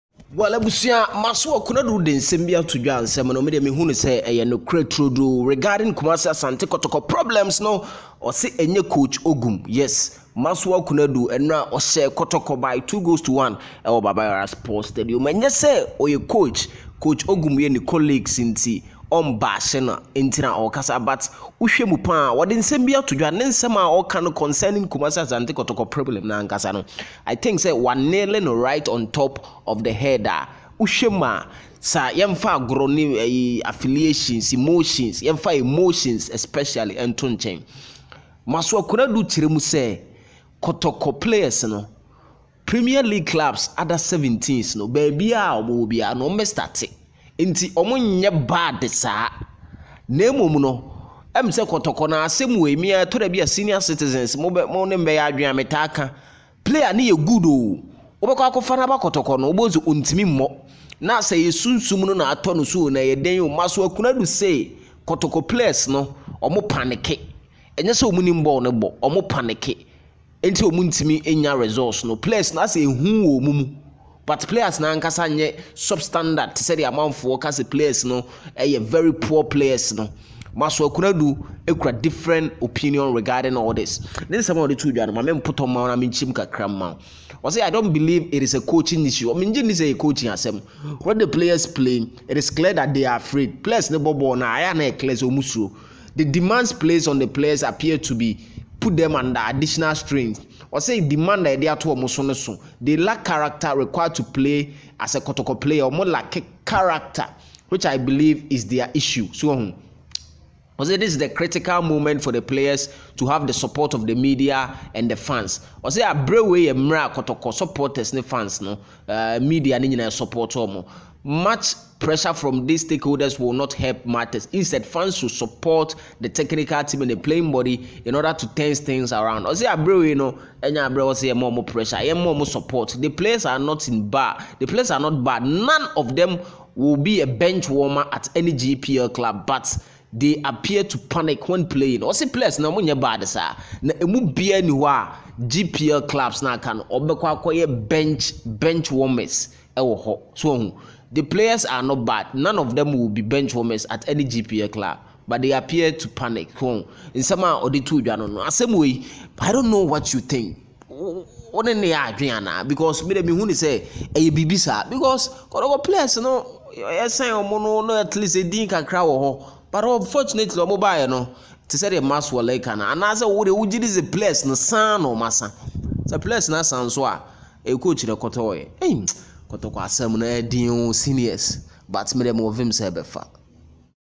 'OGUM IS NOT KOTOKO'S PROBLEM' - MAXWELL KONADU REVEALS WHY KOTOKO NOT PERFORMING
Maxwell KONADU revealed the problem of Asante Kotoko. watch video for details.